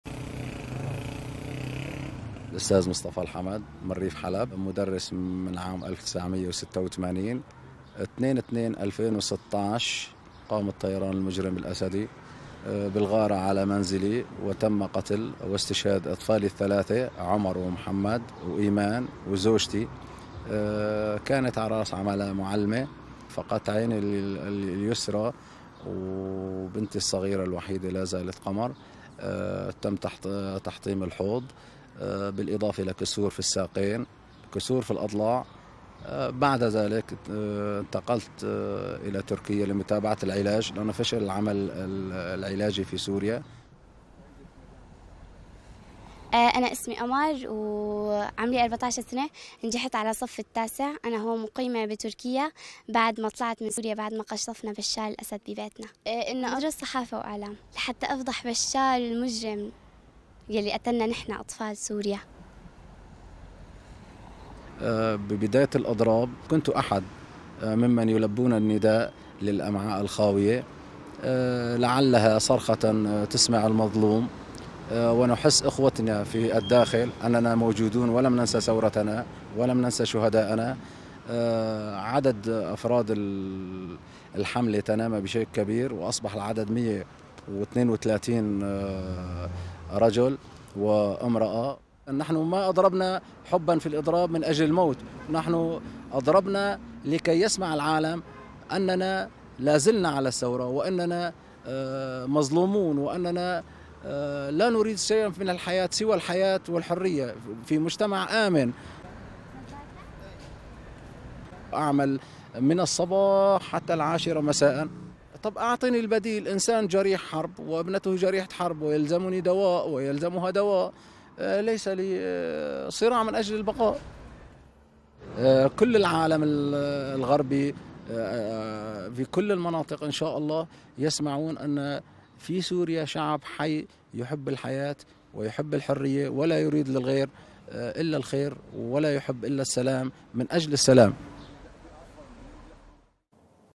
سوريون في غازي عنتاب يشاركون في حملة الأمعاء الخاوية تضامناً إدلب
فقد مصطفى زوجتَه وثلاثةً من أطفاله بعد أن قصفت طائرةٌ حربية منزلَه في عندان بريف حلب، و بقيَ على قيد الحياةِ مع ابنتِه قمر ليشاركَ في حملة الأمعاءِ الخاوية من بلاد اللجوء، تضامناً مع المدنيين تحت الحملةِ العسكريةِ المستمرة في إدلب.